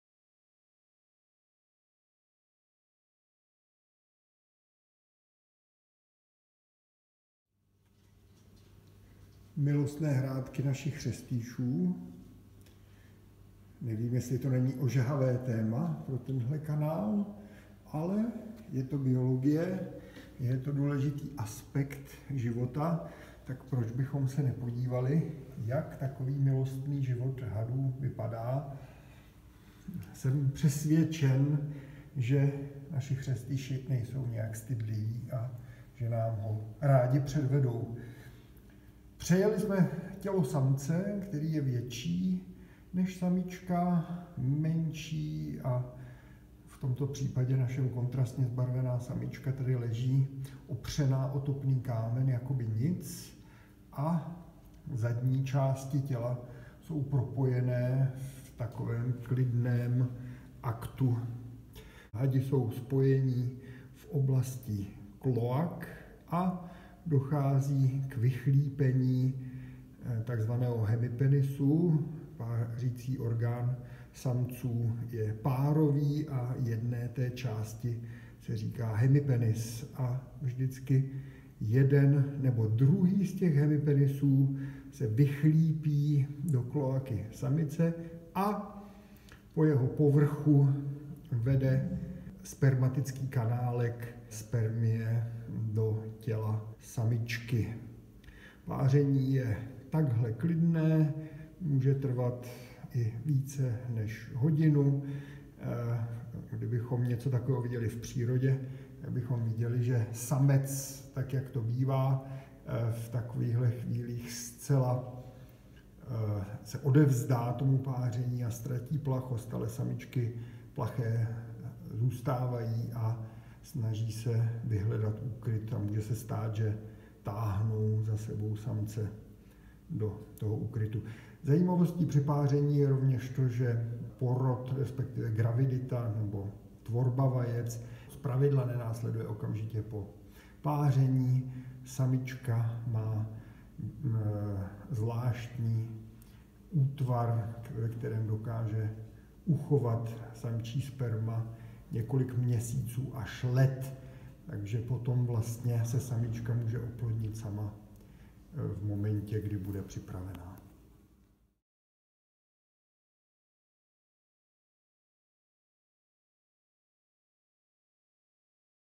Páření chřestýšů kostkovaných (Petr Velenský)
Úterý 1. prosince, video 3.
Chřestýš kostkovaný je statný had s velkou hlavou a nápadnou kresbou, která mu dala jméno. Jak vypadají jeho námluvy? Jejich průběh vám osvětlí kurátor plazů Zoo Praha!
Autor: Petr Velenský

Podpořte chov chřestýšů kostkovaných v Zoo Praha! Libovolnou částkou na ně můžete přispět zde